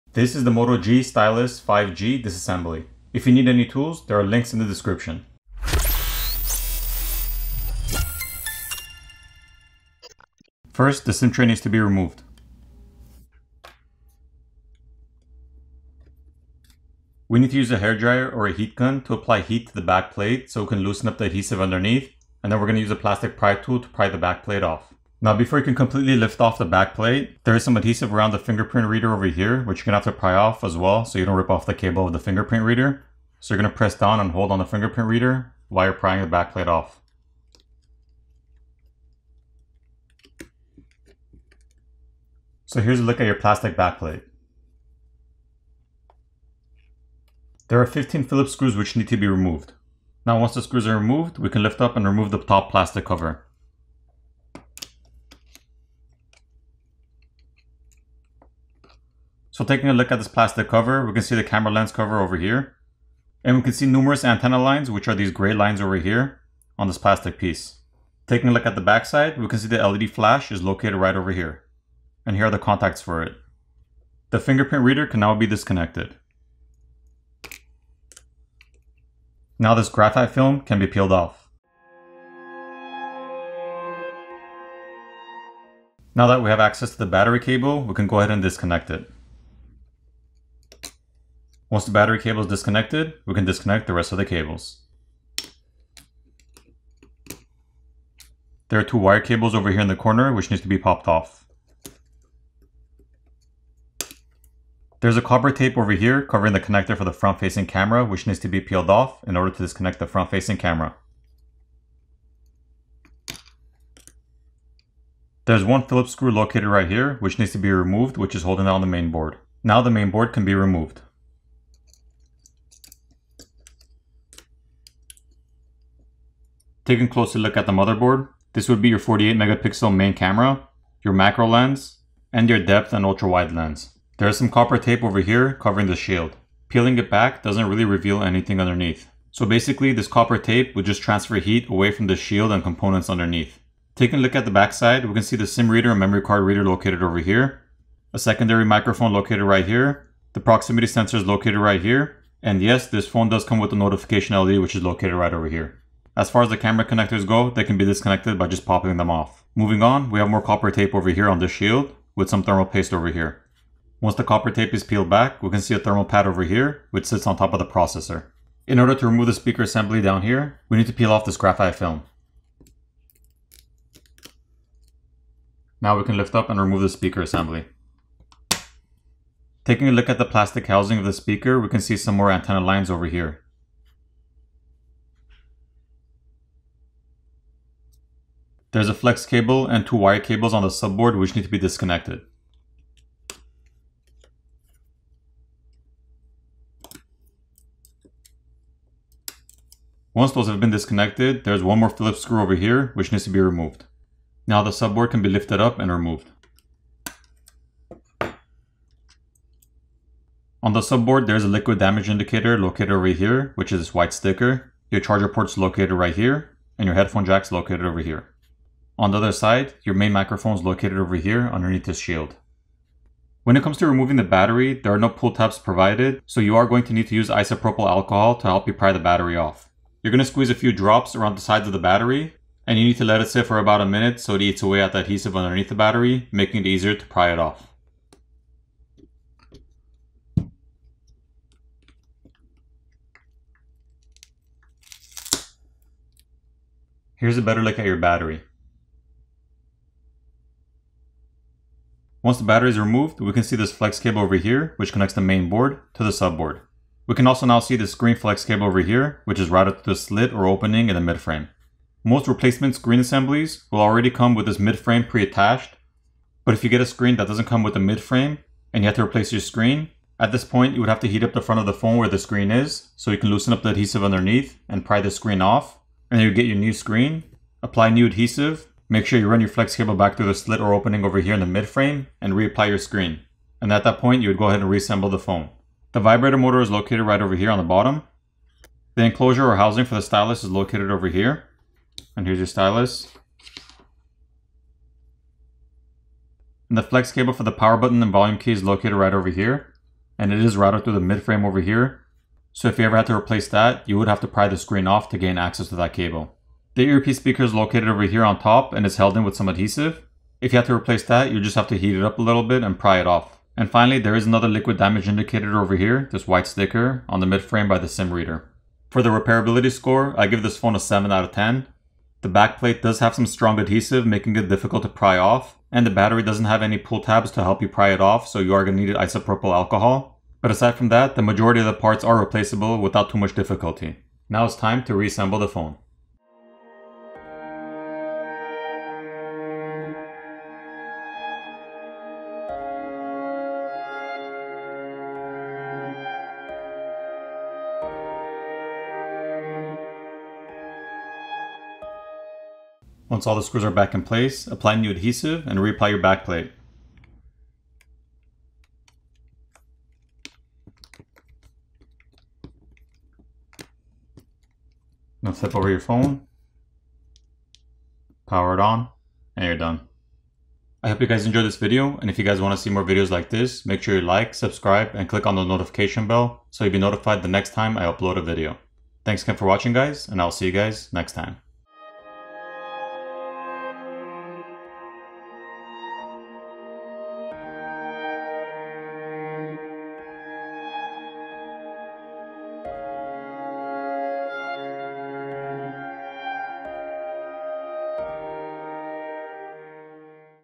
Motorola Moto G Stylus 5G 2021 Disassembly Teardown Repair Video Review
Repairability SCORE: 7/10

300LSE Adhesive